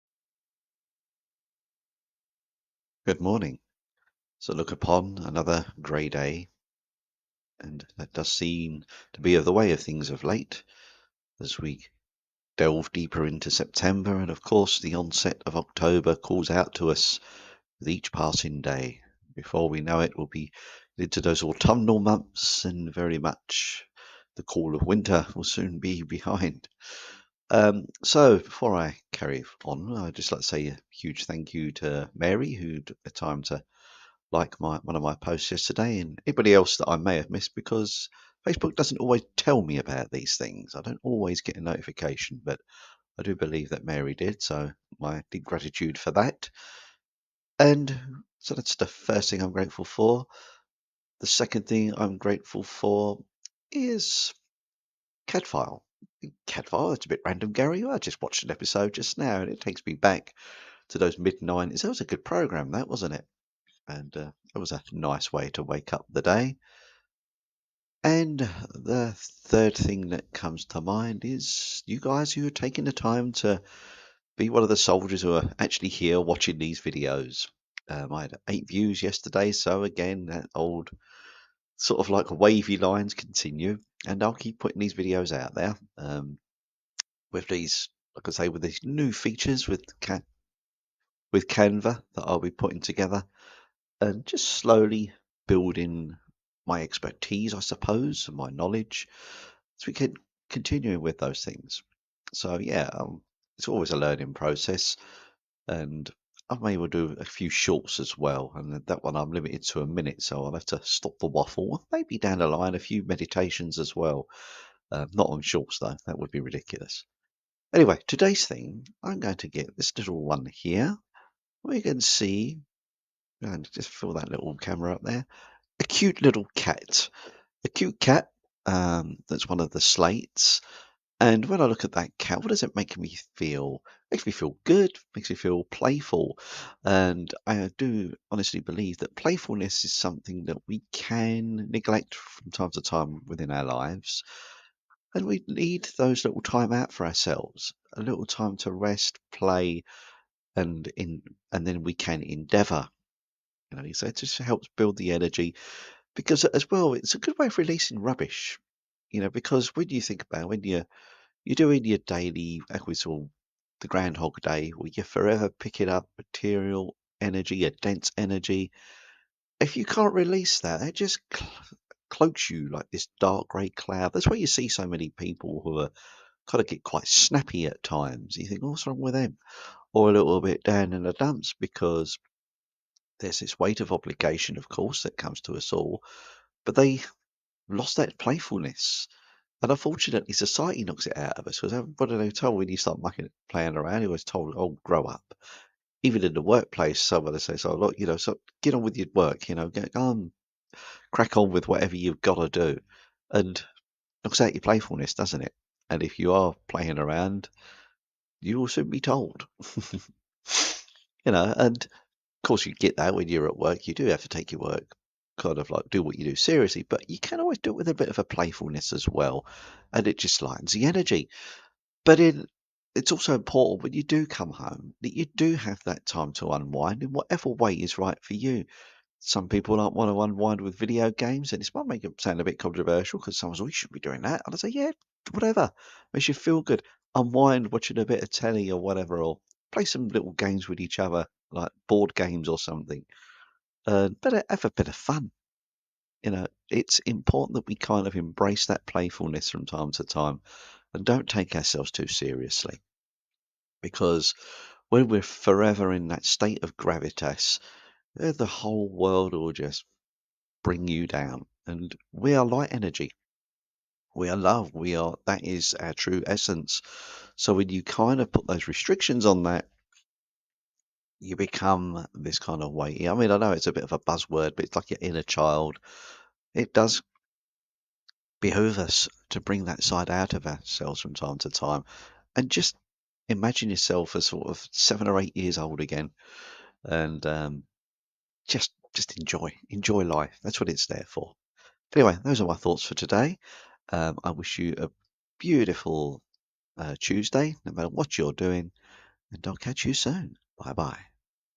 The Spiritual Magic of Play: Awakening Your Inner Child
So many of us are caught up in a serious energy that we neglect to take the time to find that playful side of us. Today, I look into that along with a fun little cat slate too.